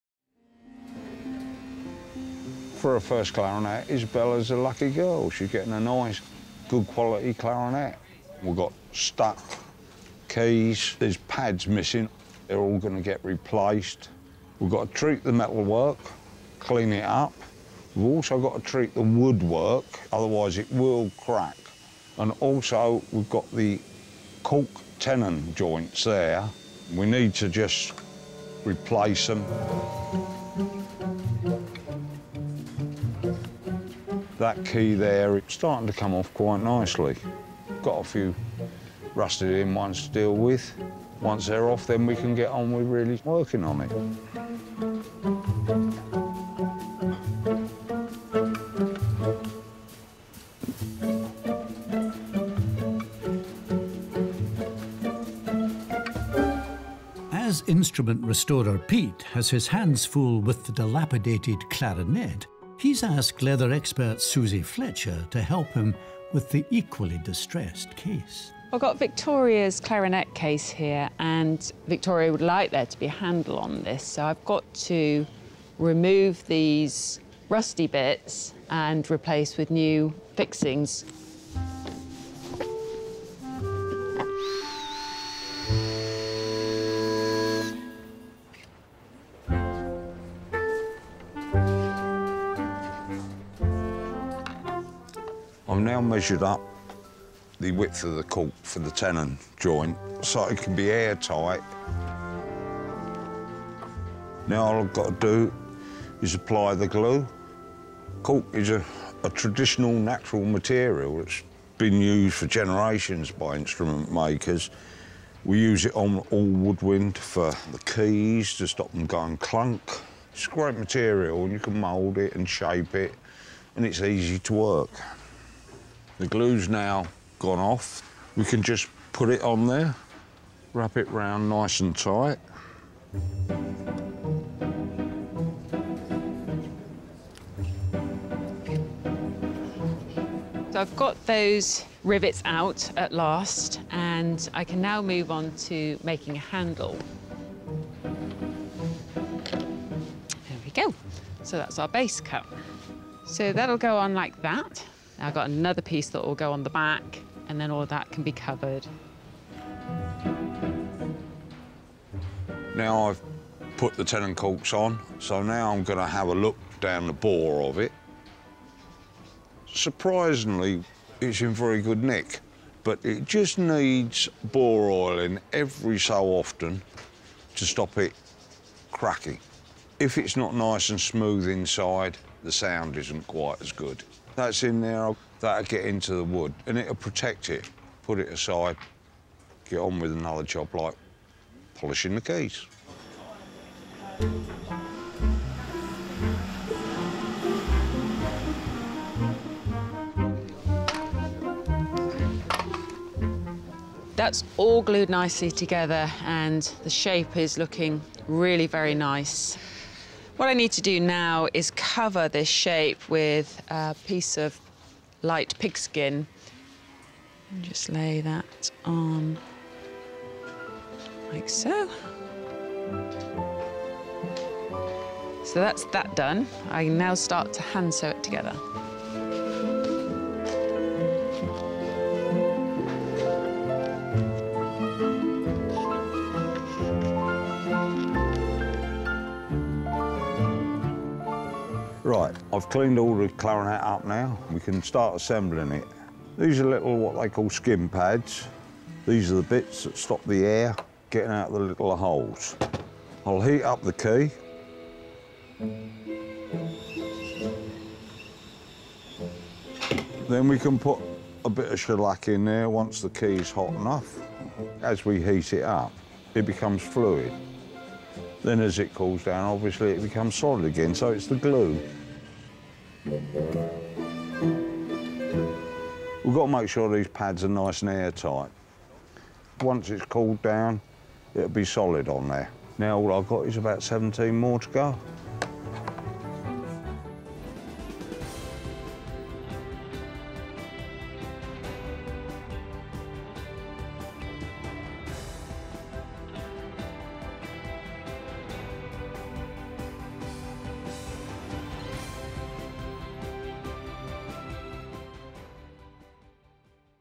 Pete and Suzie breathe new life into this clarinet!
It takes two to fix this clarinet – Pete, musical instrument expert, and Suzie, leather repair expert, who need to team up to breathe new life in this family instrument.